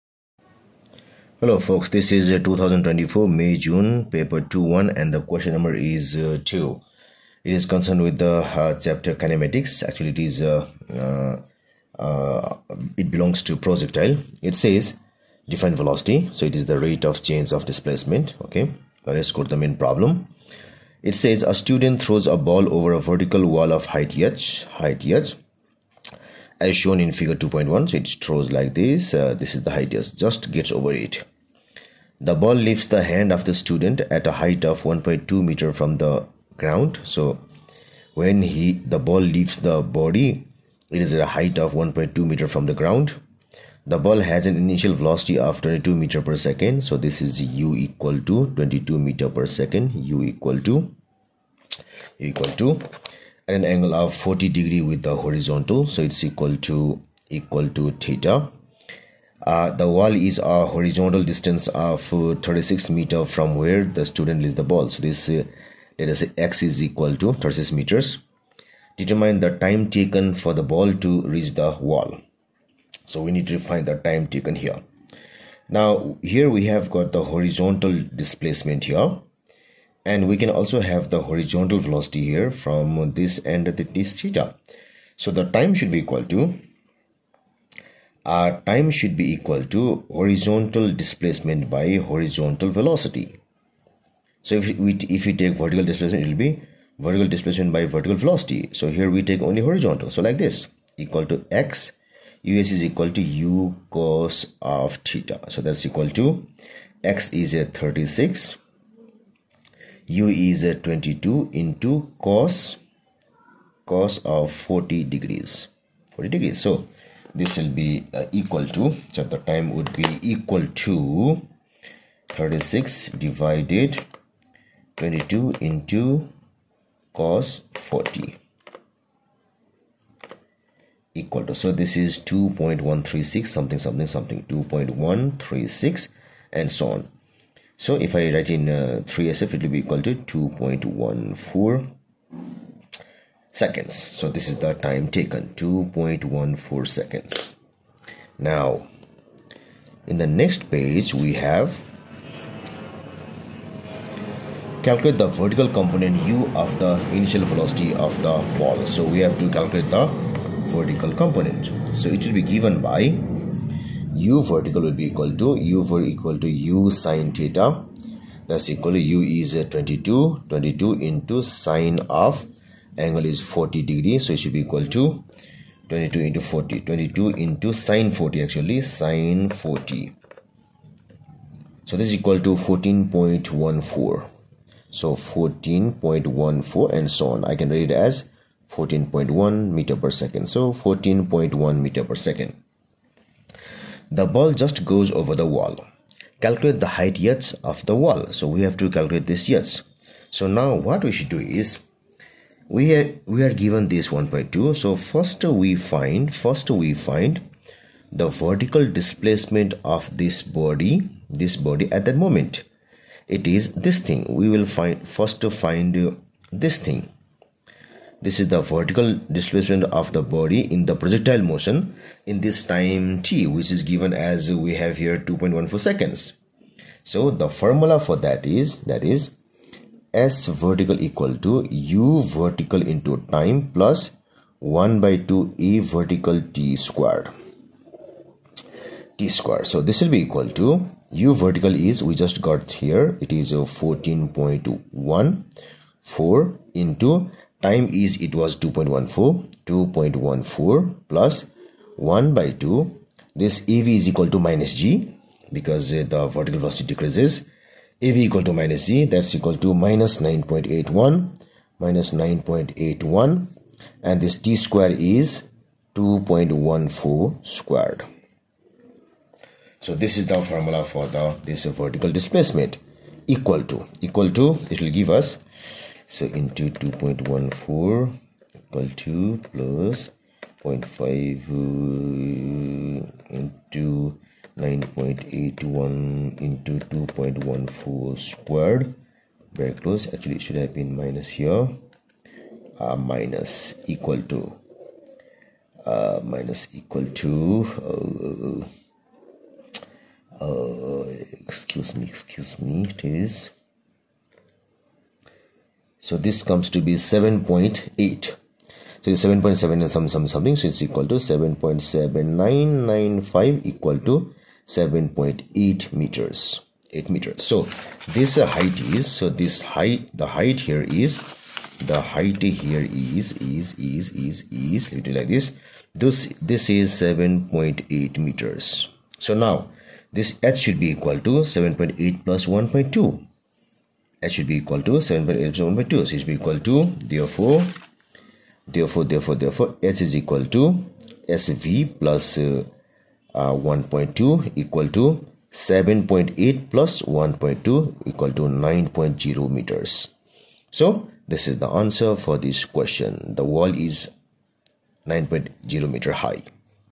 2024 CAIE AS & A level May June Physics Paper 21 Q. No. 02 (9702/21/M/J/24) by Sajit C Shakya
Solution to 2024 CAIE AS & A level May June Physics Paper 21 Q. No. 02 (9702/21/M/J/24) by Sajit Chandra Shakya from Nepal

Chapter - KINEMATICS

2 (a) Define velocity. [1]

(b) A student throws a ball over a vertical wall of height h, as shown in Fig. 2.1.
Fig. 2.1 (not to scale)

The ball leaves the hand of the student at a height of 1.2m above the horizontal ground.

The ball has an initial velocity of 22ms–1 at an angle of 40° to the horizontal.

The wall is a horizontal distance of 36m from where the student releases the ball.

Air resistance is negligible.

(i) Determine the time taken for the ball to reach the wall.
time taken = s [2]

(ii) Calculate the vertical component u of the initial velocity of the ball.
u = .ms–1 [1]

(iii) The ball just goes over the wall.

Calculate the height h of the wall.
h = .m [3]